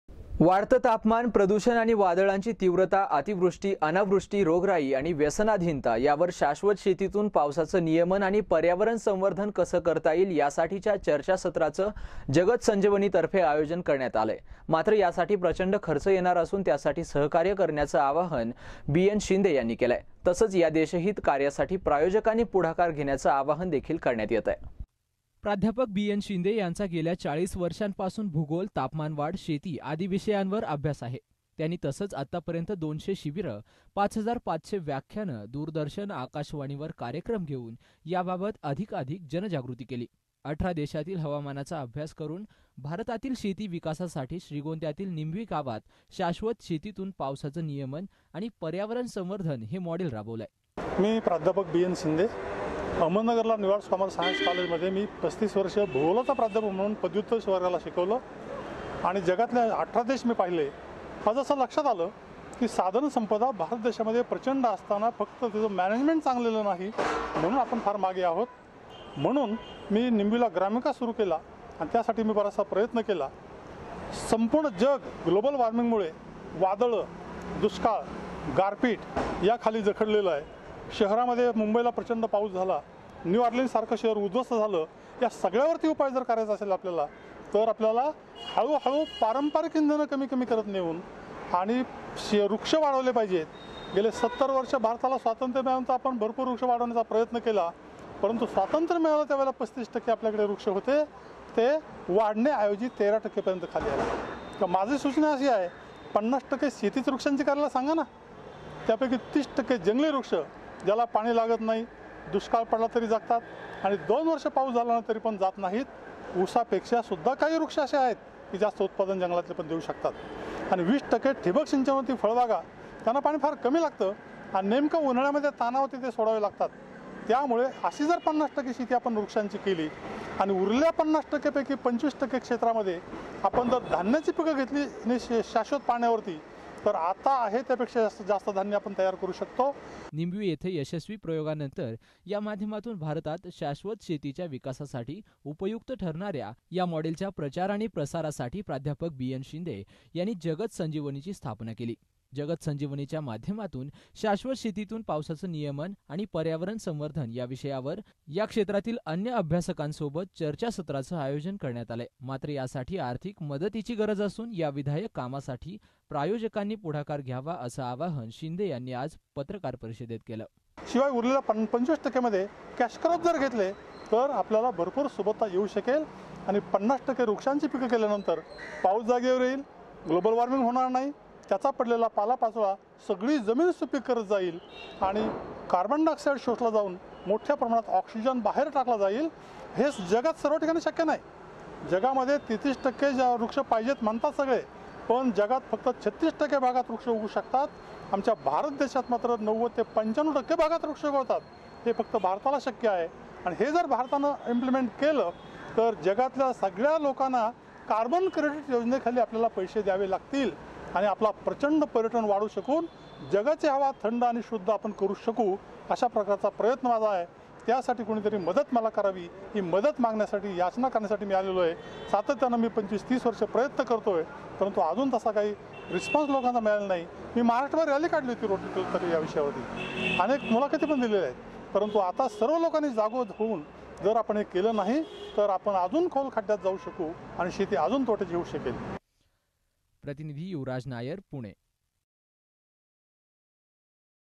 शाश्वत शेतीतून पर्जन्य नियमन आणि पर्यावरण संवर्धन चर्चाचसत्रास सहकार्य करण्याचे आवाहन
वाढते तापमान, प्रदूषण आणि वादळांची तीव्रता, अतिवृष्टी, अनावृष्टी, रोगराई आणि व्यसनाधिनता यावर शाश्वत शेतीतून पावसाचे नियमन आणि पर्यावरण संवर्धन कसे करता येईल यासाठीच्या चर्चासत्राचं जगत संजीवनीतर्फे आयोजन करण्यात येत आहे. मात्र, यासाठी प्रचंड खर्च येणार असून त्यासाठी सहकार्य करण्याचे आवाहन जगत संजीवनीचे प्राध्यापक बी.एन शिंदे यांनी केले आहे. तसेच या देशहित कार्यासाठी प्रायोजकांनी पुढाकार घ्यावा, असंही आवाहन त्यांनी केलय.